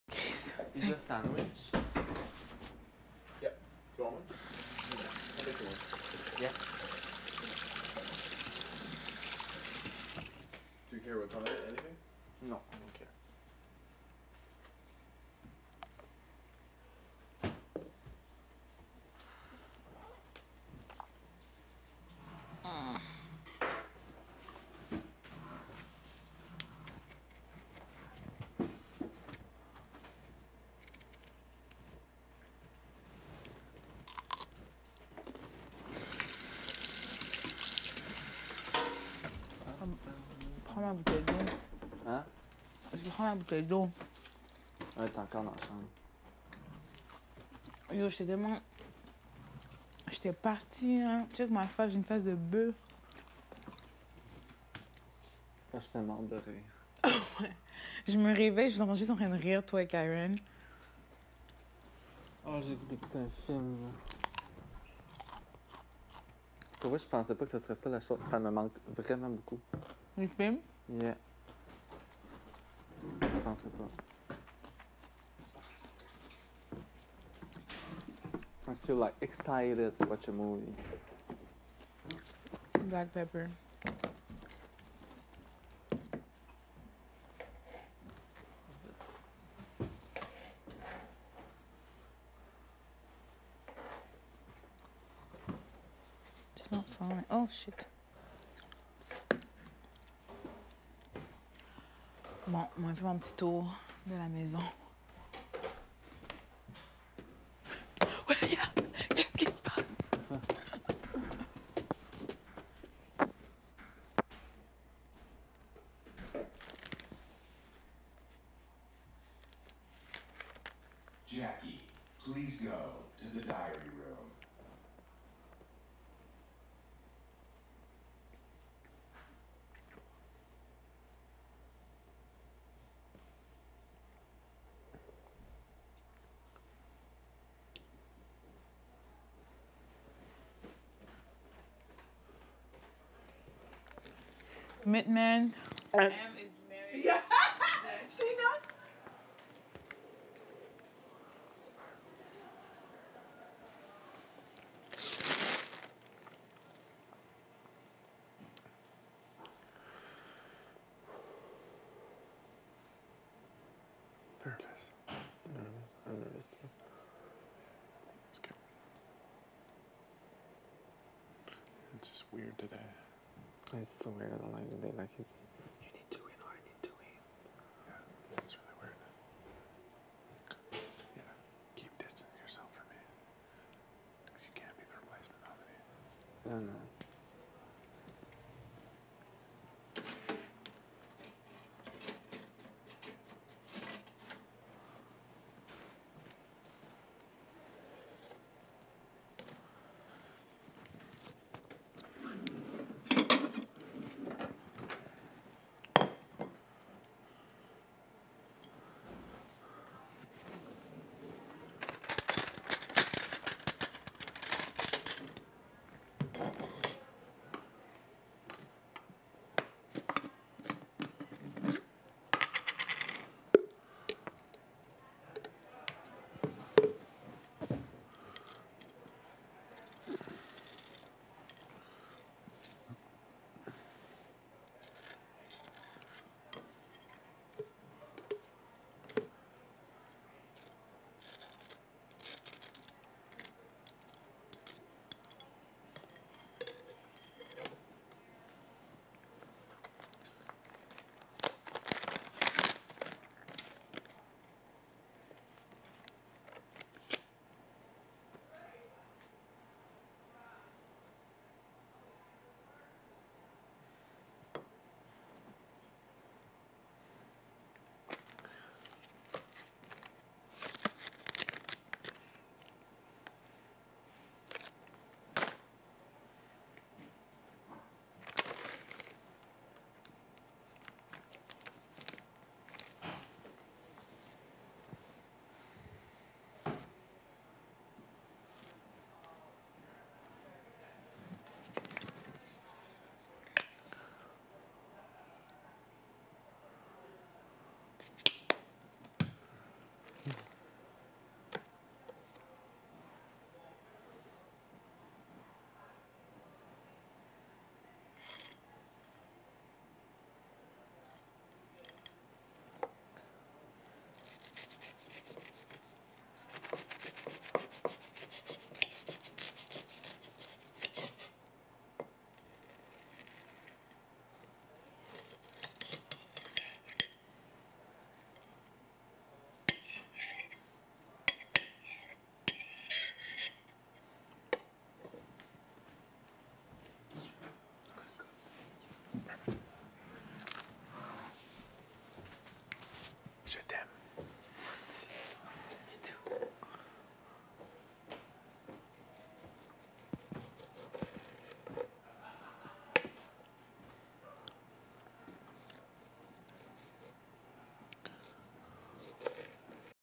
Day 43 - 4/22 4:10p - Kev makes Will food, whispers he's scared & "I love you" in French.
Kevin: nervous
Will: your nervous? I'm nervous too.
Kevin: scared
Kevin: it's just weird today
Will: so weird, I don't like the day, like you need to win or I need to win.
Kevin: yeah it's really weird
Kevin: yeah keep distancing yourself from me, because you can't be the replacement nominee.
Will: No no
Kevin: je t'aime
Will: you too